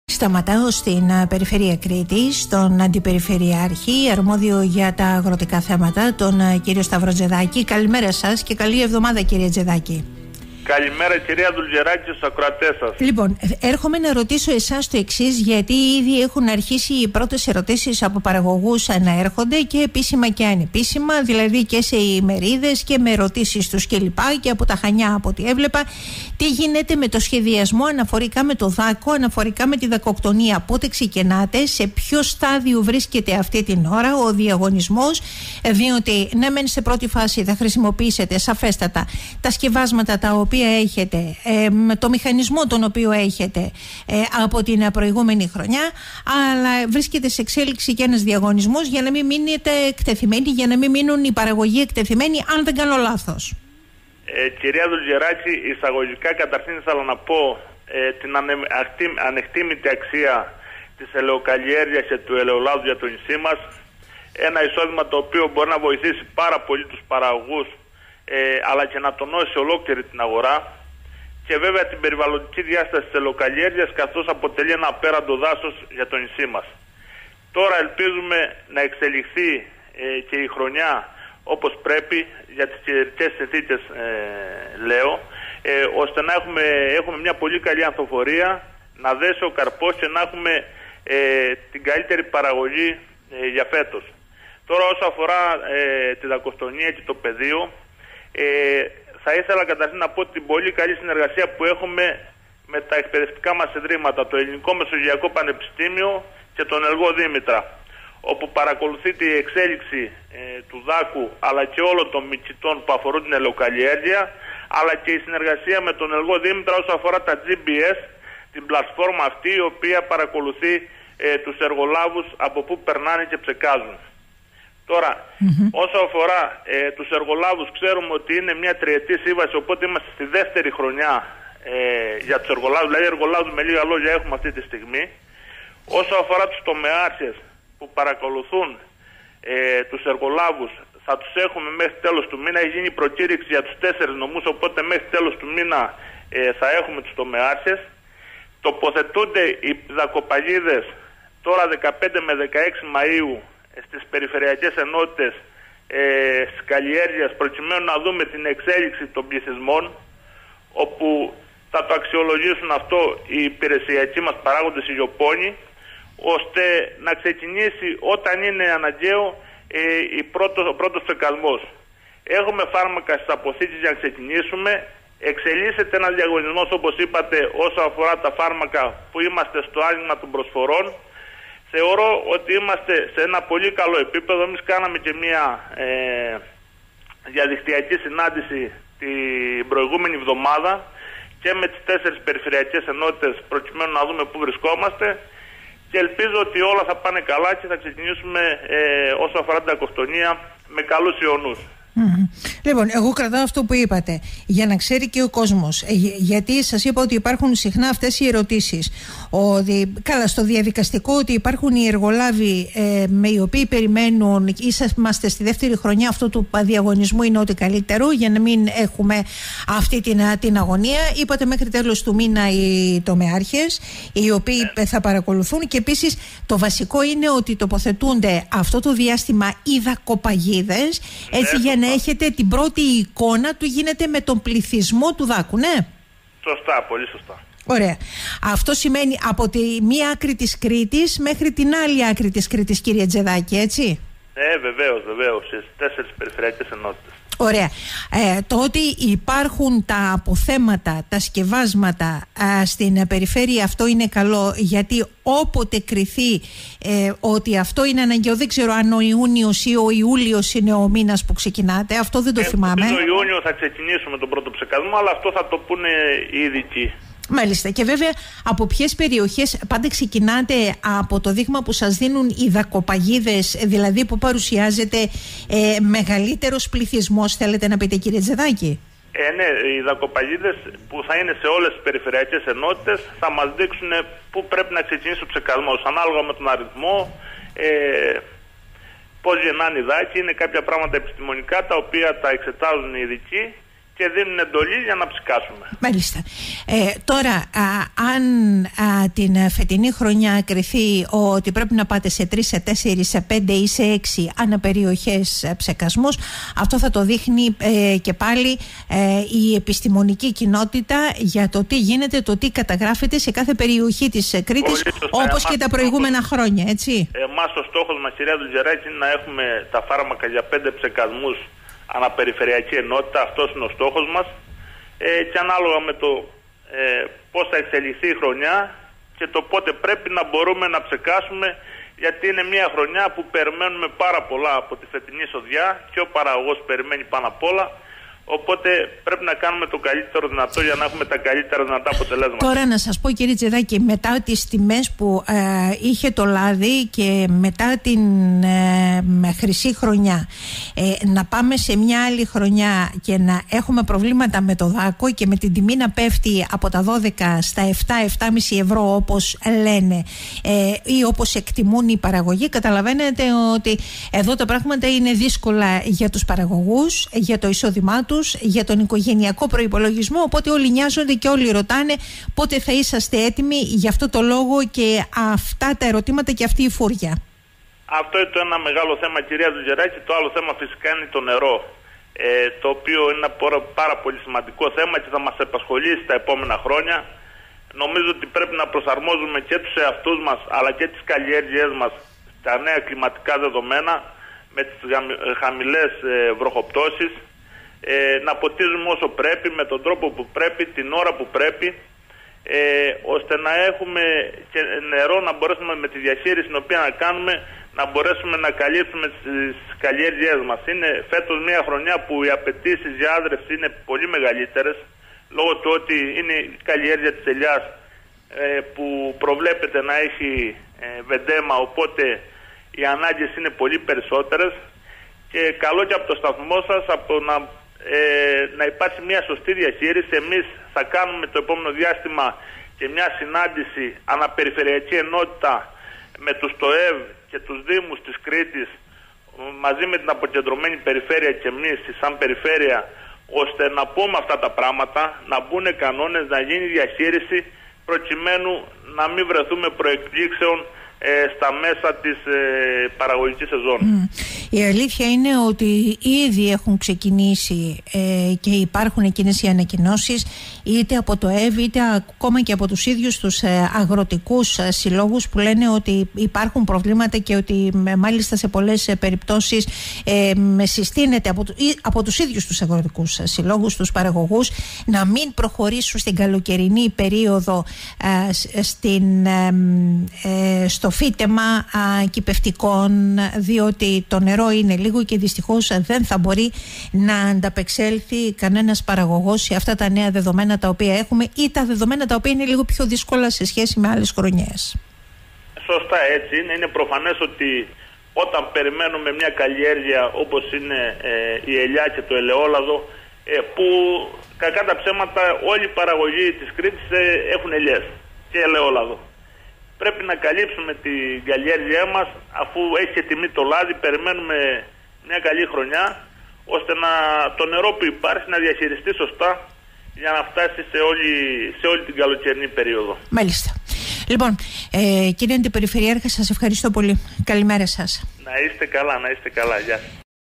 Ξεκινά η καταγραφή του πληθυσμού του Δάκου σε όλη την Κρήτη
Εντός του μήνα ολοκληρώνεται η τοποθέτηση των δακοπαγίδων προκειμένου η Περιφέρεια με τα Επιστημονικά Ιδρύματα να ξέρουν πότε κι από πού θα ξεκινήσεις η δακοκτονία.

‘’Οι εργολάβοι είναι στη θέση τους, τέλος του μήνα θα έχουν οριστεί κι οι Τομεάρχες, τα σκευάσματα υπάρχουν και τρέχει ο διαγωνισμός για τη συμπλήρωσή τους λέει στο Ράδιο 98.4 ο Αντιπεριφερειάρχης Αγροτικής Οικονομίας Σταύρος Τζεδάκης.